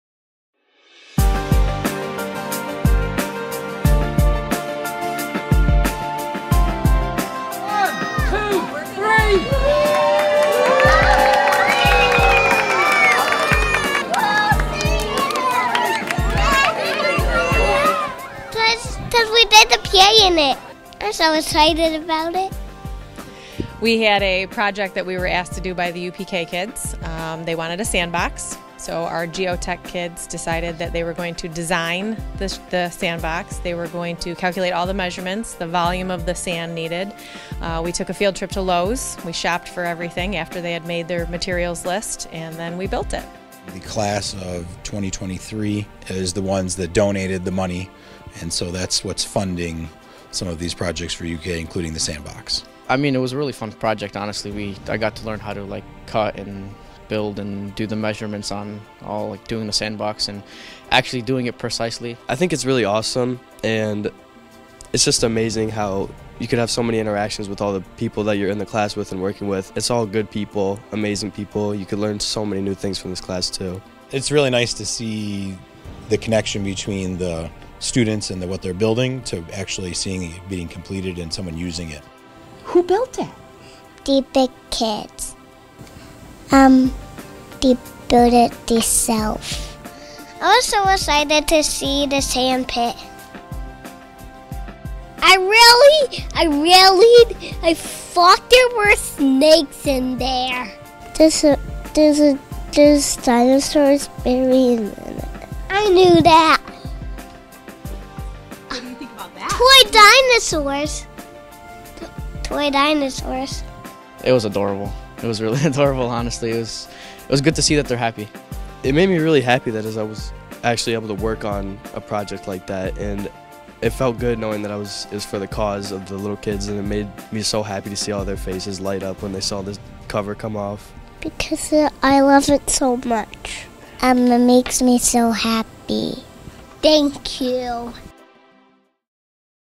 New Pre-K Sandbox
Webster Schroeder GeoTech students unveil the new sandbox they designed and built for the Pre-K students.